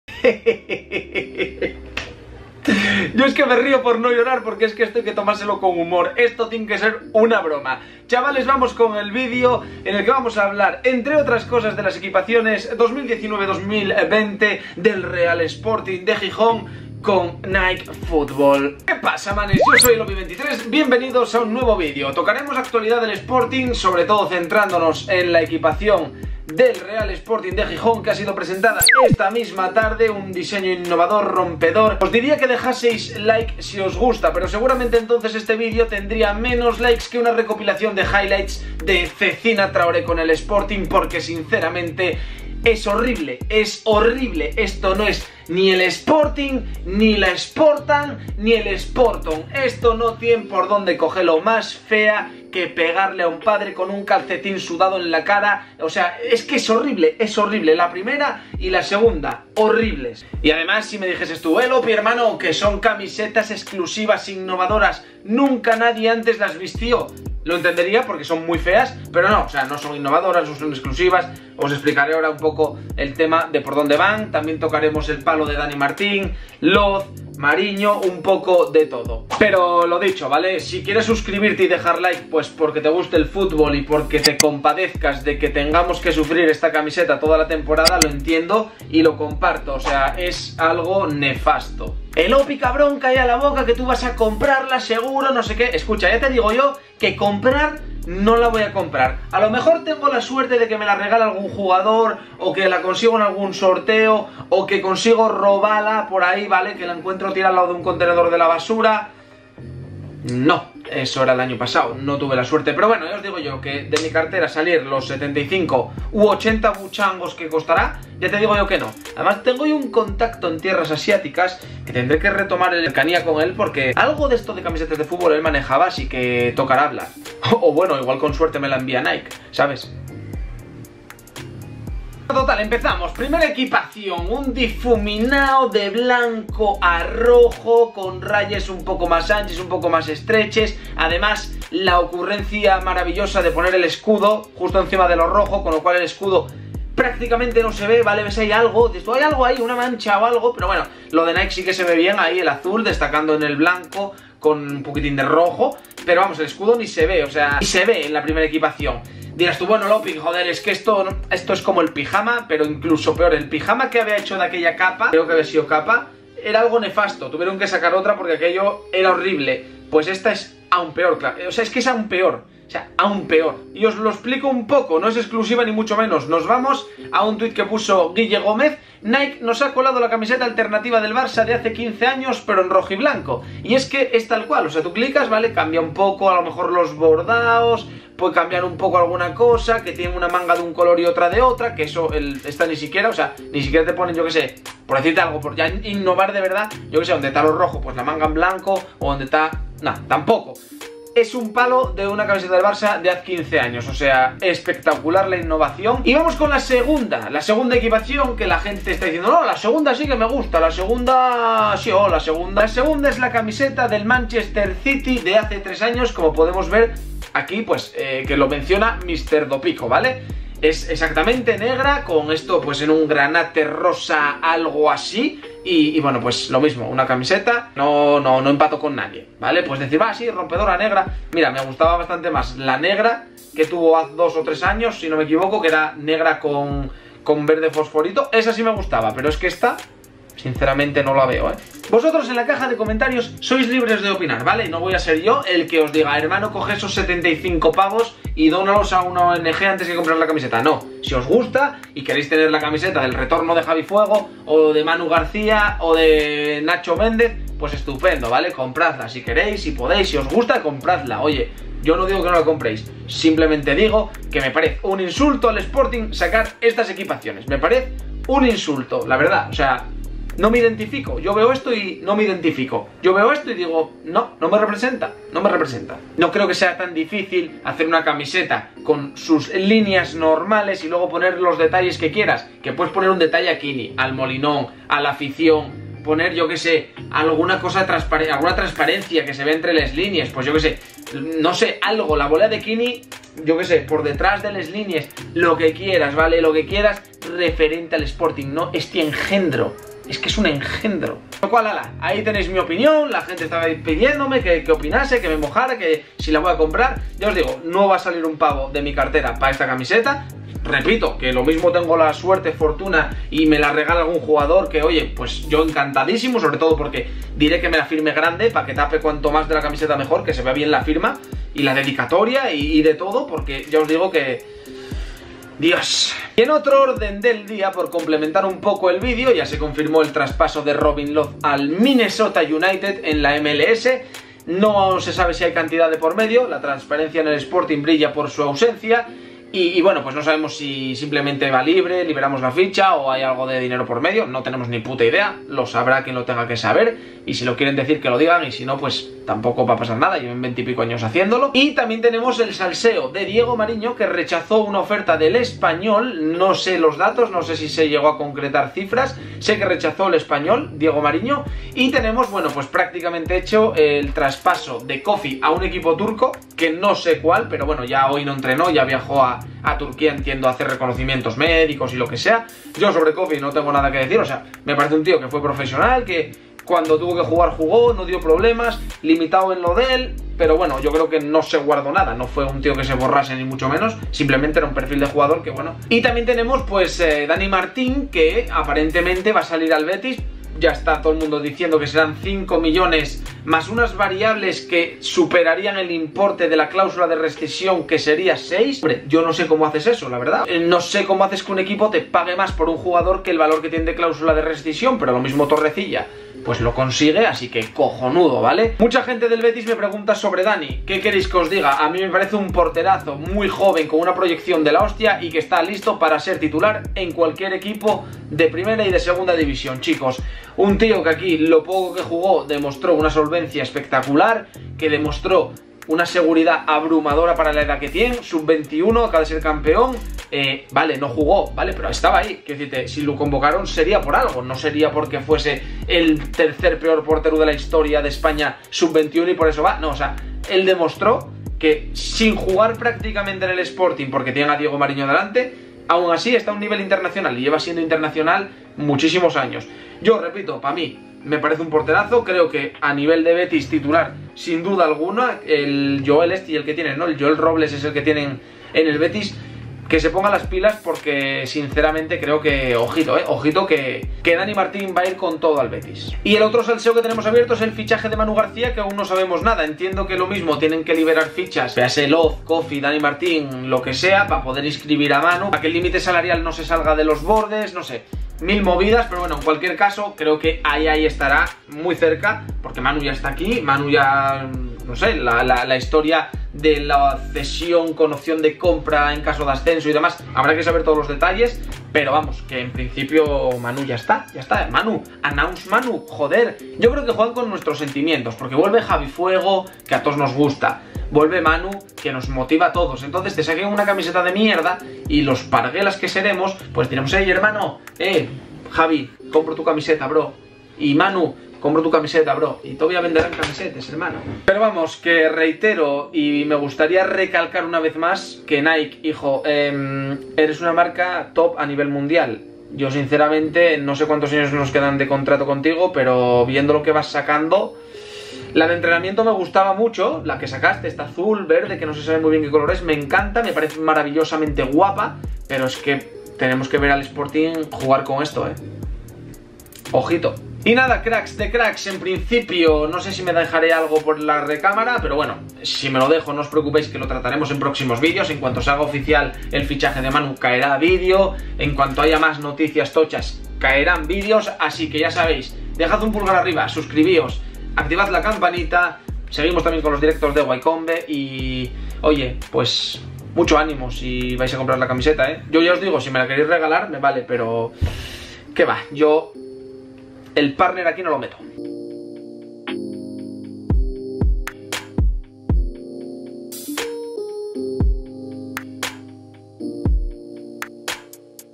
NUEVA EQUIPACIÓN 2019/2020 del REAL SPORTING DE GIJÓN *mi opinión*
Hoy se ha presentado la nueva equipación del Real Sporting de Gijón 2019/2020 de la mano de Nike, ahí va mi opinión.

🤑¿Quieres patrocinar mi canal de Youtube para apoyarme? Sigue estos pasos:

2º) Sigue los pasos ;)